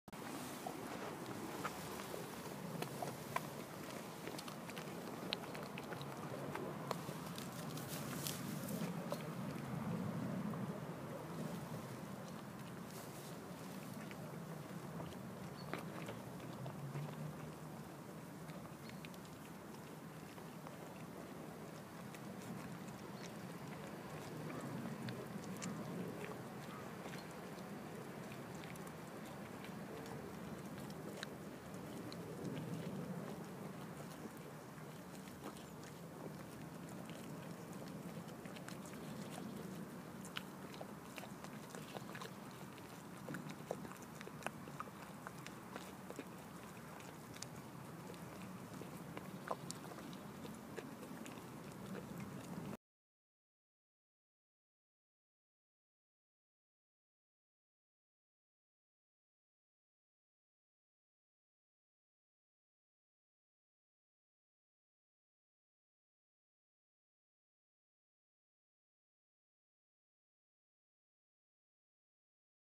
Die Ziege Birgit kaut Brot
Wir sehen und hören, wie die Deutsche Edelziege Birgit ein Stück hartes Brot kaut. Ziegen haben keine obereren Schneidezähne, sodass das sich das Kauen größerer Stücke als schwierig erweist. Das Team von Kaugeräusche.de besuchte den Hof Stallgespräch Alfter und nahm Kaugeräusche verschiedener Tiere auf.